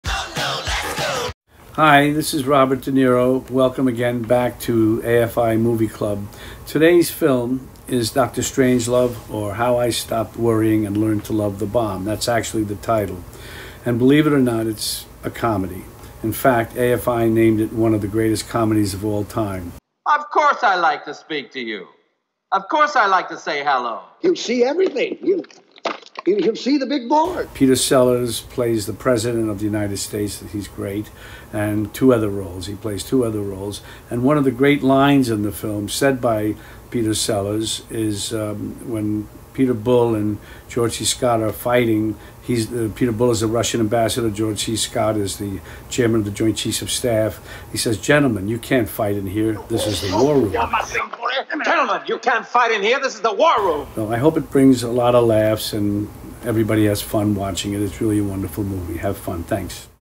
Robert De Niro on Dr. Strangelove
Robert De Niro reacts to Stanley Kubrick's 1964 masterpiece Dr. Strangelove or: How I Learned to Stop Worrying and Love the Bomb.